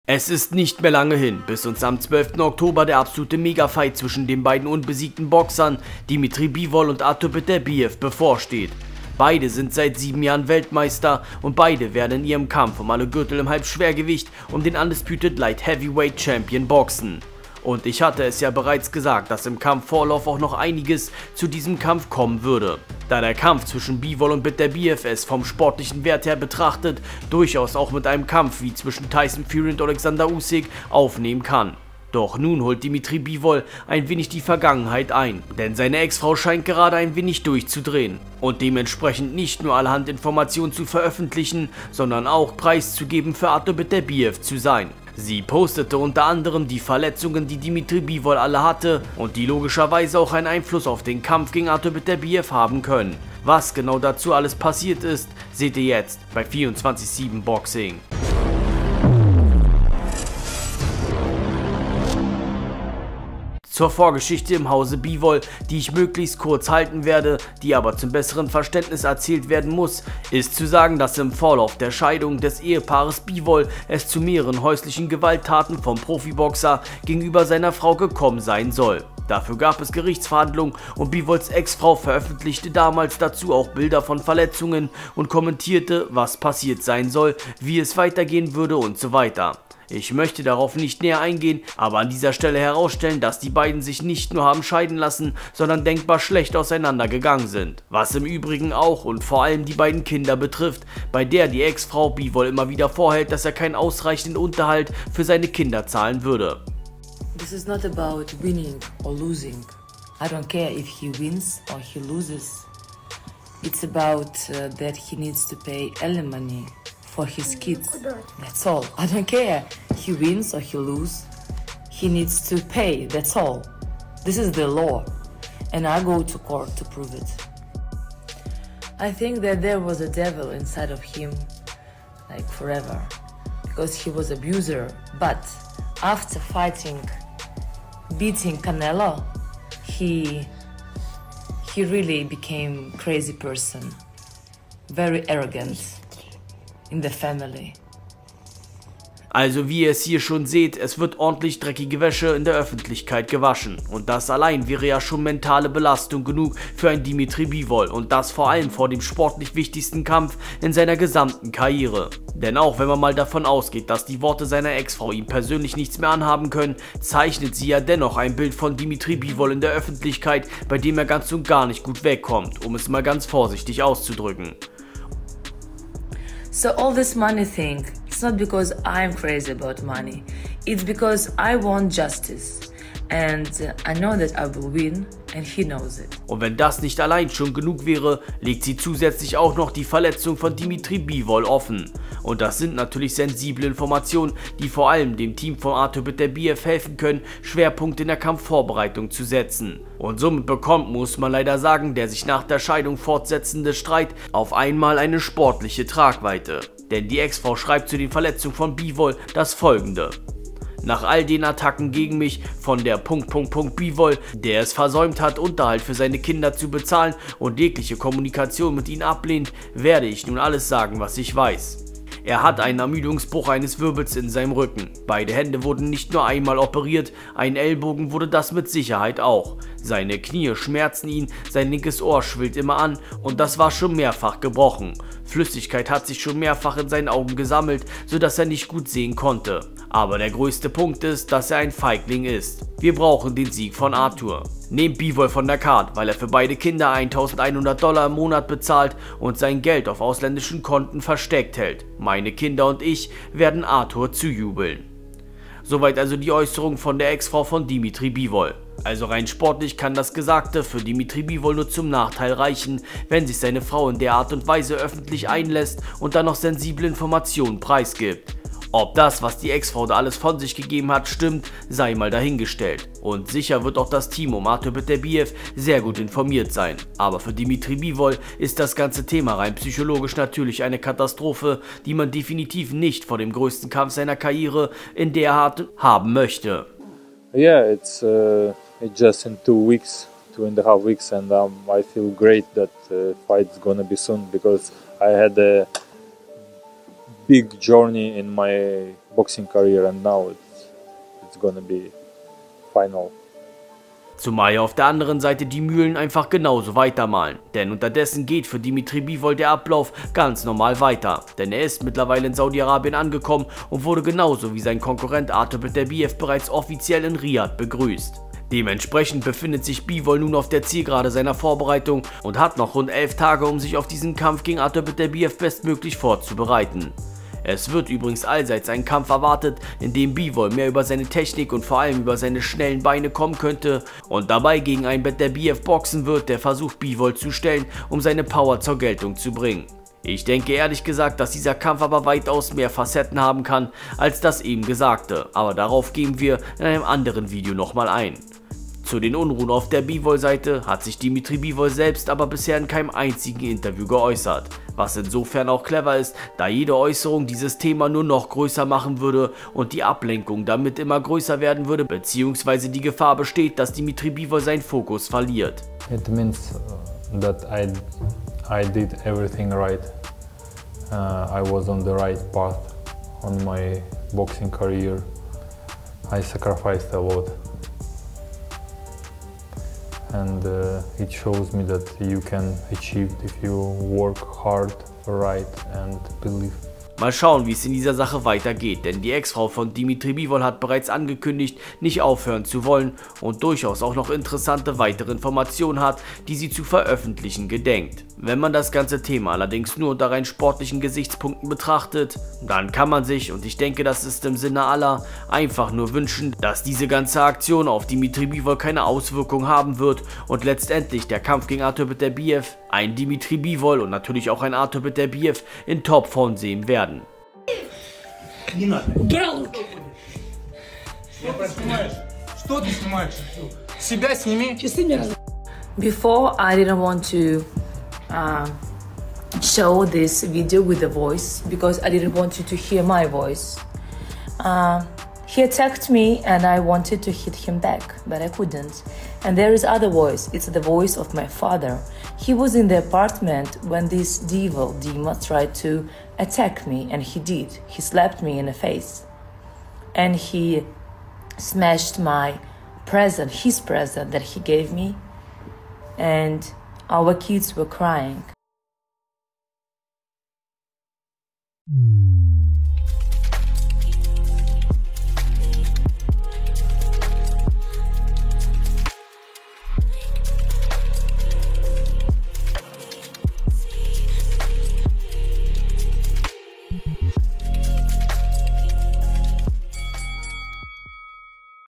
BIVOLS EX-FRAU sorgt für miesen SKANDAL vor dem großen Kampf und stellt ihn ÖFFENTLICH bloß!!!
Es ist nicht mehr lange hin bis uns am 12. Oktober der absolute Mega fight zwischen den beiden unbesiegten Boxern Dimitry Bivol und Artur Beterbiev bevorsteht. Beide sind seit 7 Jahren Weltmeister und beide werden in ihrem Kampf um alle Gürtel im Halbschwergewicht um den undisputed Light Heavyweight Champion boxen. Und ich hatte es ja bereits gesagt, dass im Kampfvorlauf auch noch einiges zu diesem Kampf kommen wird, der es vom sportliche Wert mit dem Kampf zwischen Fury und Usyk durchaus aufnehmen kann. Doch nun holt Dimitry Bivol ein wenig die Vergangenheit, denn seine Ex- frau scheint gerade ein wenig durchzudrehen und nicht nur veröffentlicht für Artur Beterbiev zu sein, sondern auch die Verletzungen von Bivol posted, die einen Einfluss auf den Kampf haben können. Was genau dazu passiert ist, seht ihr jetzt bei 247 Boxing.